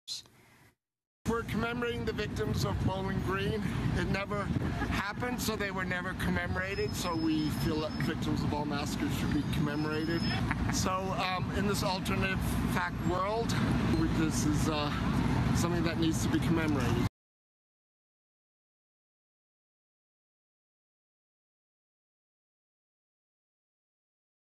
Vigilia en “conmemoración” de las víctimas imaginarias de un ataque terrorista que nunca sucedió
El viernes 3 de febrero en Nueva York, los manifestantes se congregaron para llevar a cabo una parodia de una vigilia en “conmemoración” de las víctimas imaginarias de un ataque terrorista en Bowling Green, Kentucky, que nunca sucedió, y que fue citado por la consejera del presidente, Kellyanne Conway, para intentar justificar la prohibición de Trump contra los musulmanes. Los manifestantes se reunieron frente a la estación del metro de Bowling Green en Manhattan para criticar las afirmaciones de Conway.